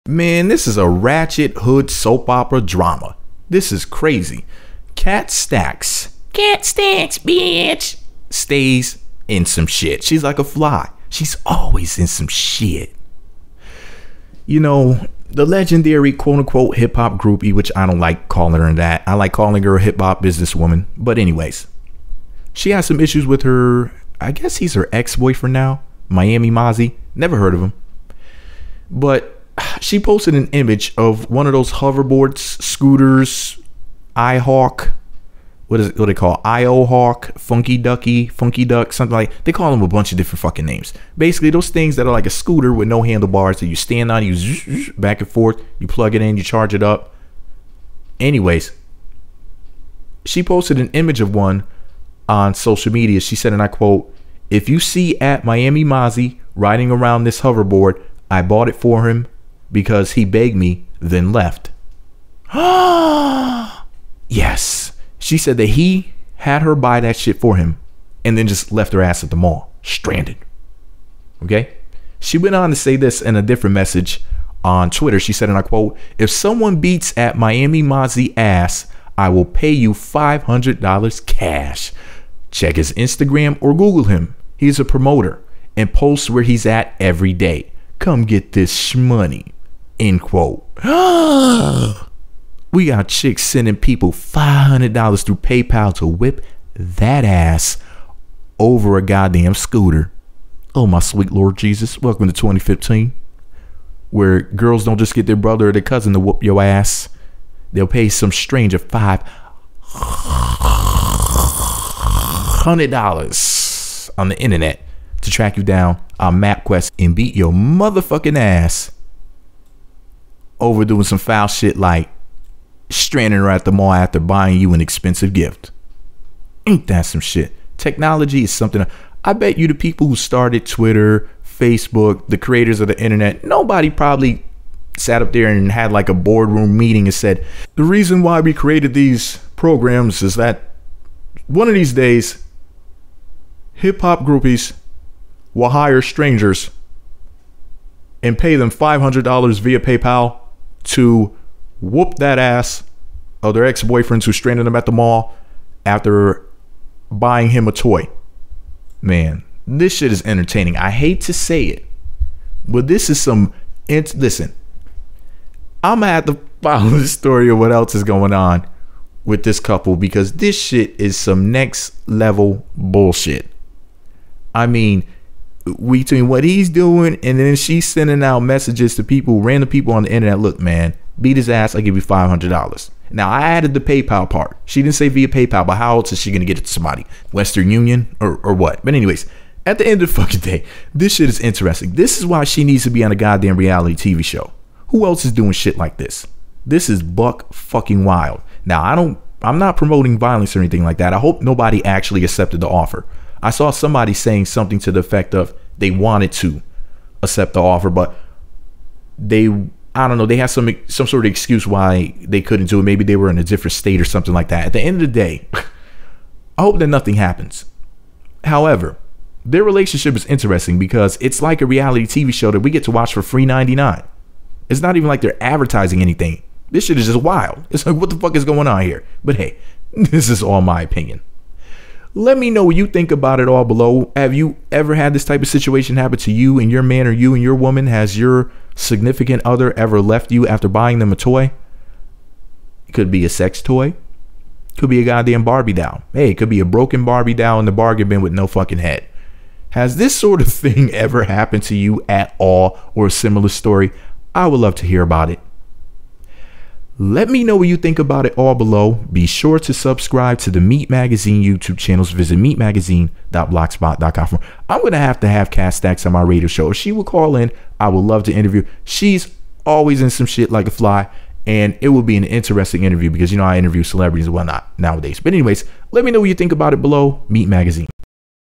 KatStacks boyfriend Miami Mazi has her buy hoverboard then leaves!! Offers reward to beat him up!
Kat Stacks boyfriend Miami Mazi uses her to buy hover board then leaves her stranded at the MALL! KatStacks offers $500 reward for someone to beat him up!